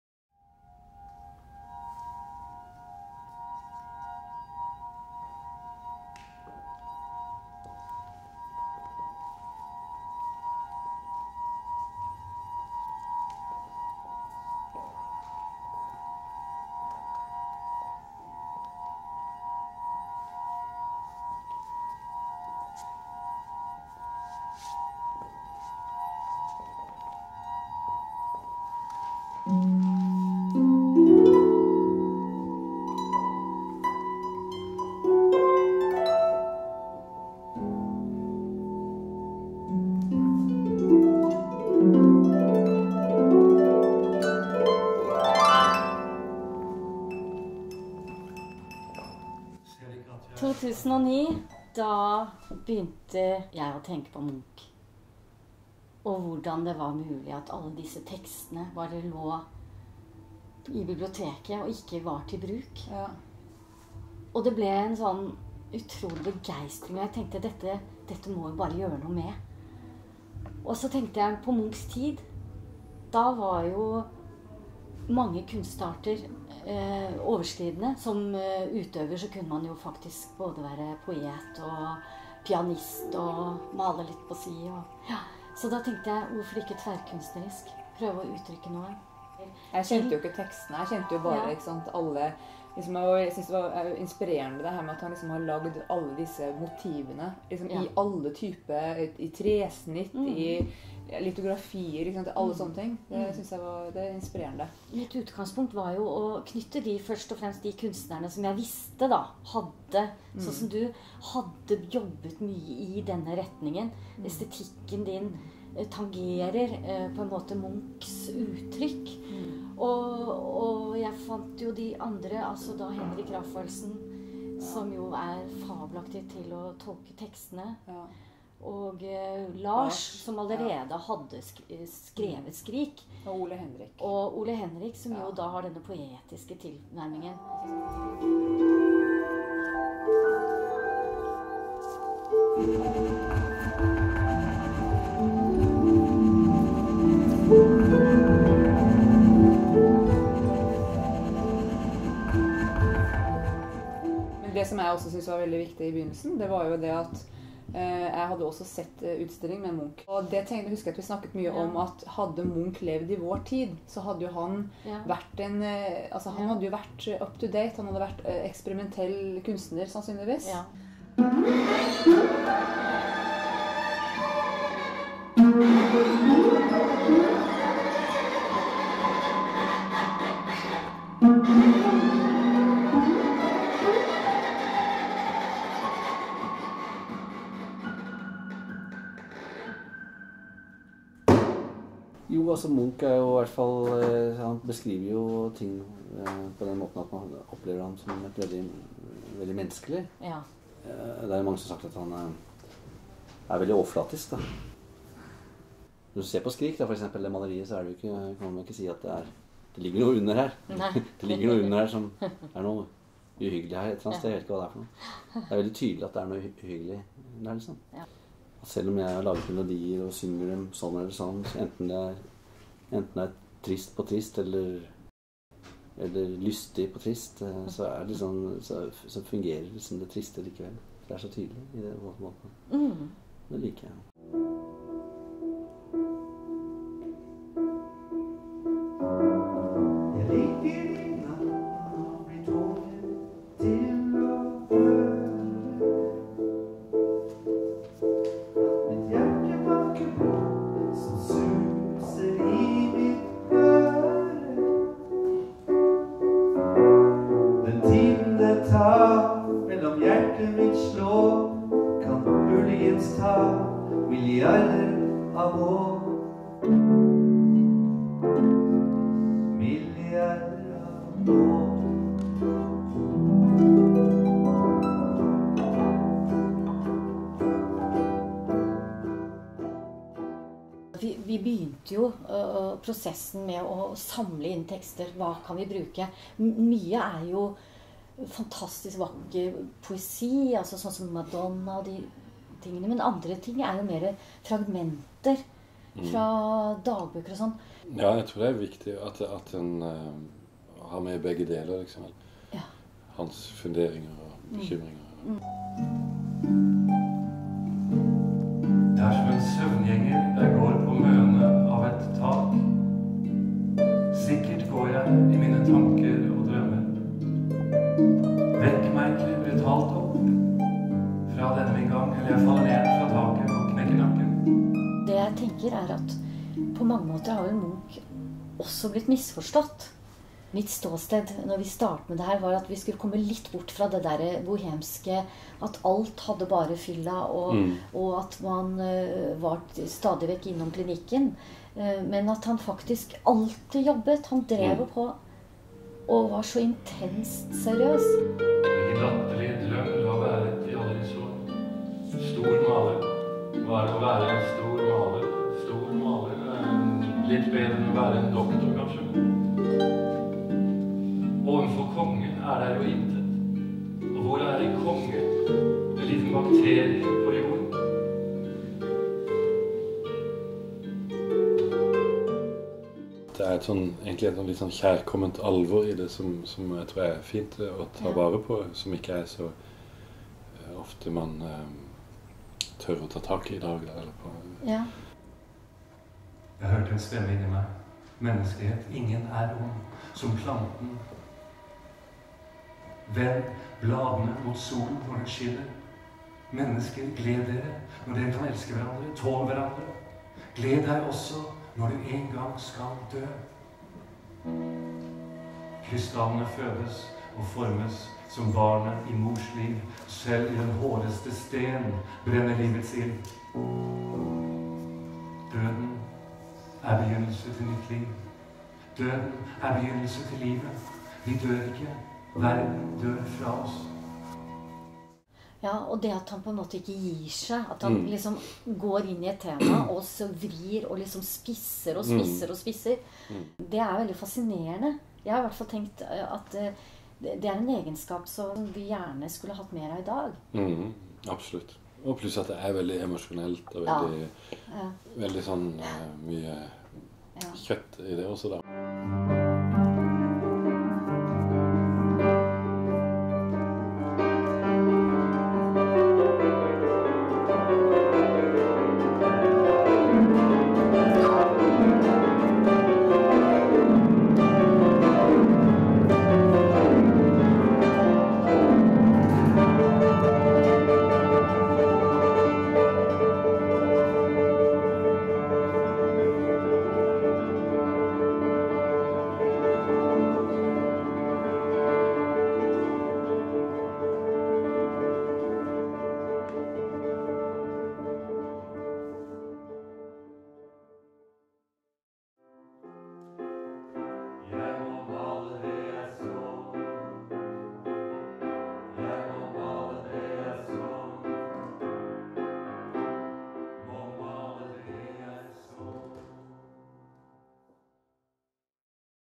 MUNCH PERFORMANCE:HJERTEBLOD-LIFEBLOOD live in New York
Meet the artists in the Munch performance Lifeblood in New York november 2013.
Ellen Sejersted Bødtker, idea´and artistic leader
Ella Fiskum, coreographer
Lars Lillo-Stenberg,artist
Henrik Rafaelsen, actor